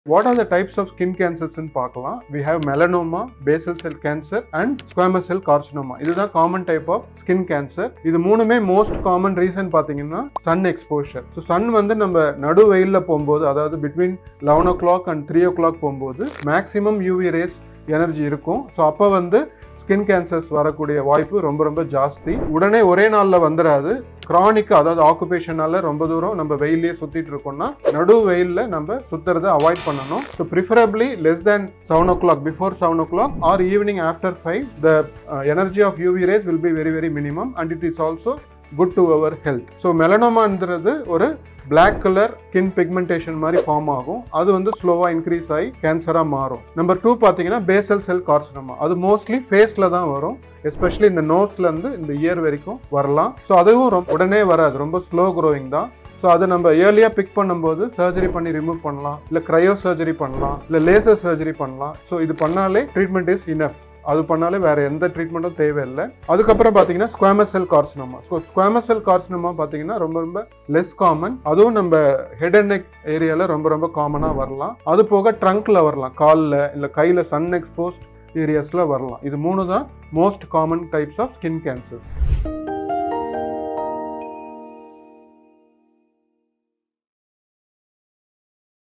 What are the types of skin cancer? | தோல் புற்றுநோயின் வகைகள் என்ன?
Sun exposure can silently damage your skin - not just today, but over years!

Do you know the three most common types of skin cancers and how to spot them early? Watch this reel to understand the difference between melanoma, basal cell carcinoma, and squamous cell carcinoma, and learn why sun timing matters for your health

This 30 seconds could save your skin - or even your life